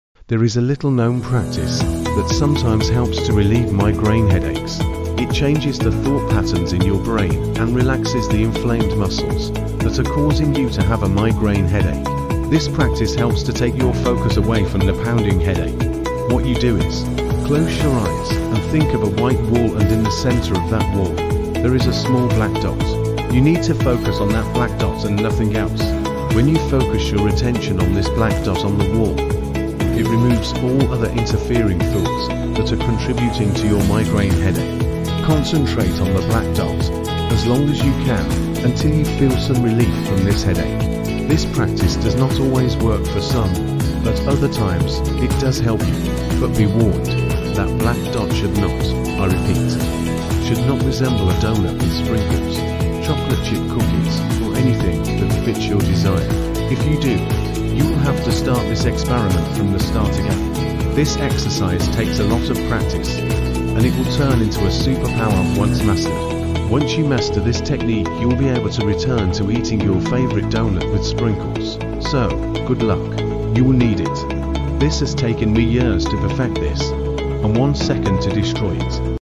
Weird and strange ways to rid of headaches.
Is it possible to relieve headaches with meditation?